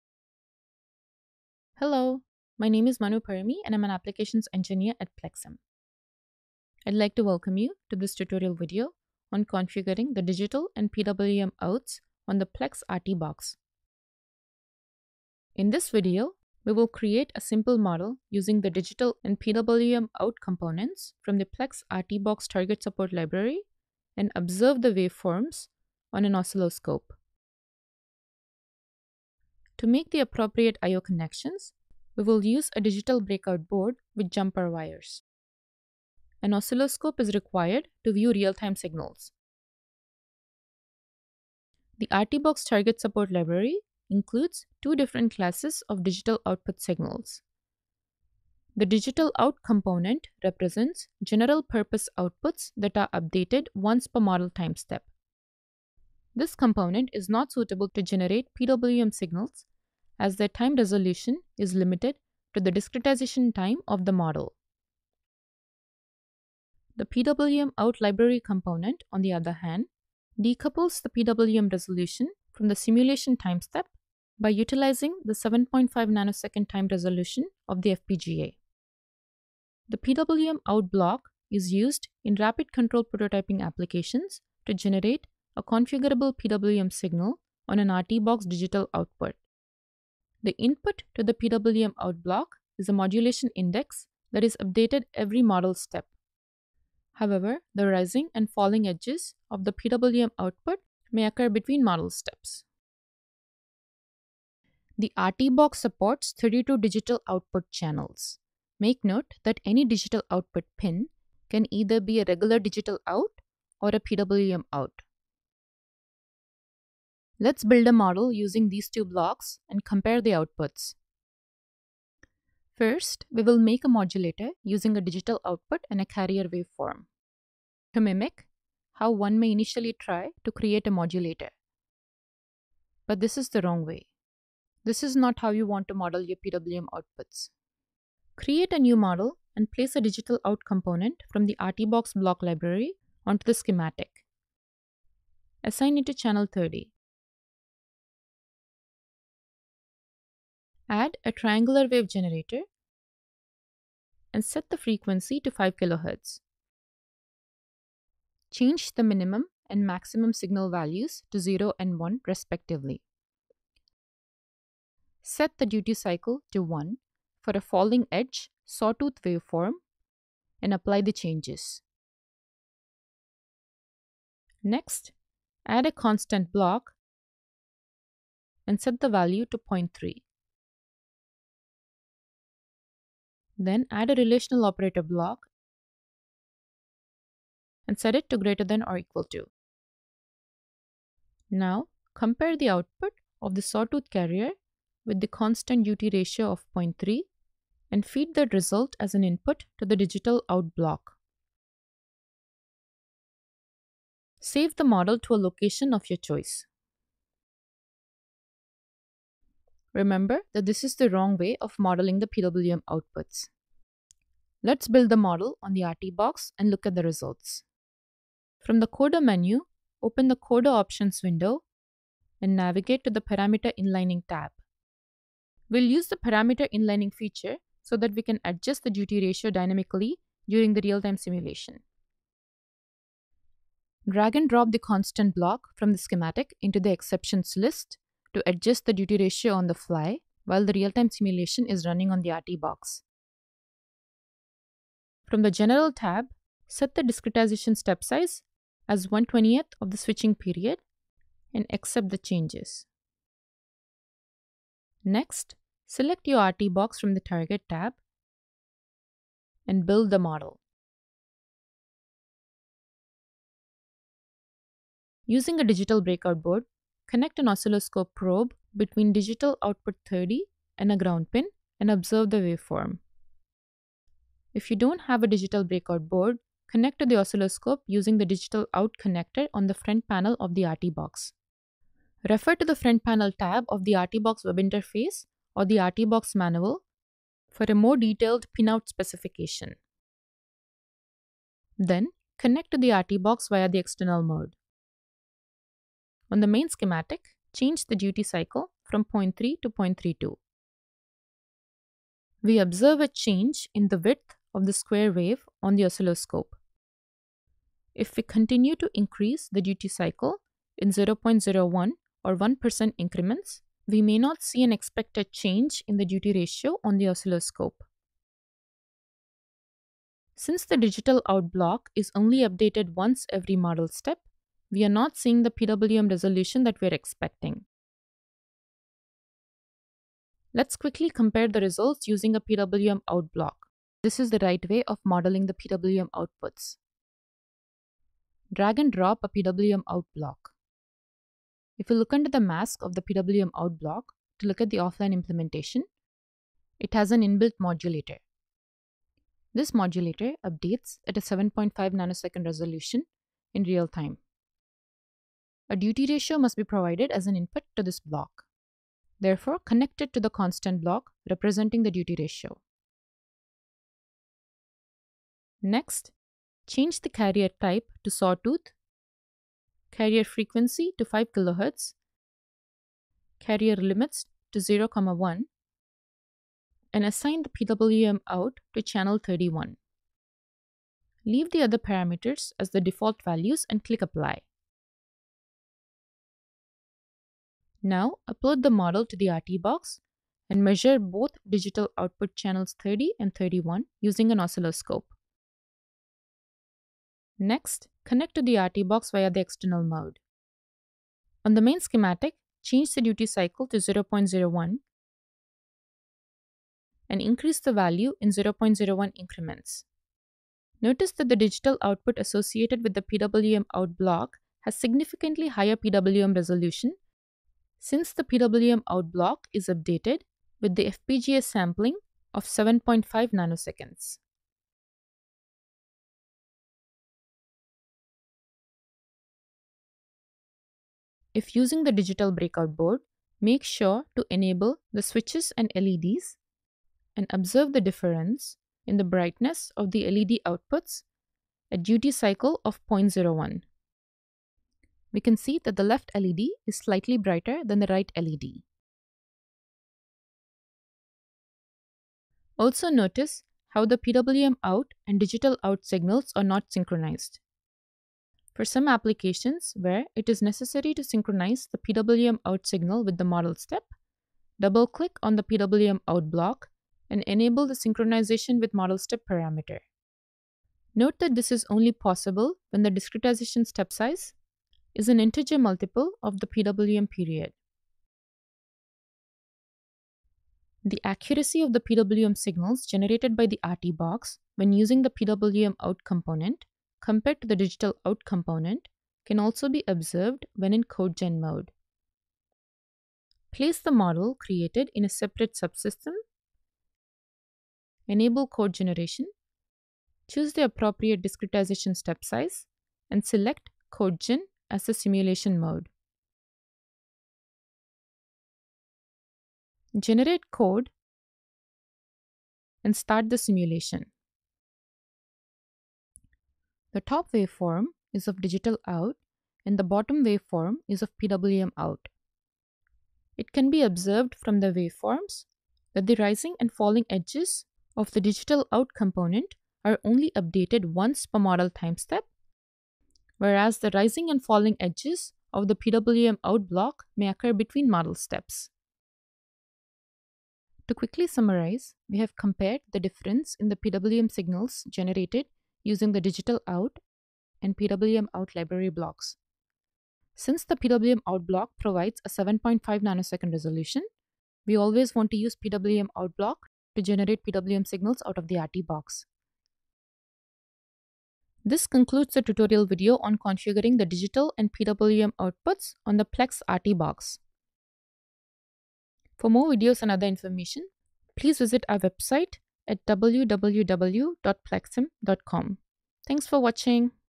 Configuring Digital and PWM Outs on PLECS RT Box 1
Watch this tutorial video to learn how to configure the Digital and PWM outputs on the PLECS RT Box 1. The PWM Out block is used in Rapid Control Prototyping applications to generate a configurable PWM signal on an RT Box digital output.

For support with setting up the RT Box, and for other RT Box tutorial videos, please follow the playlist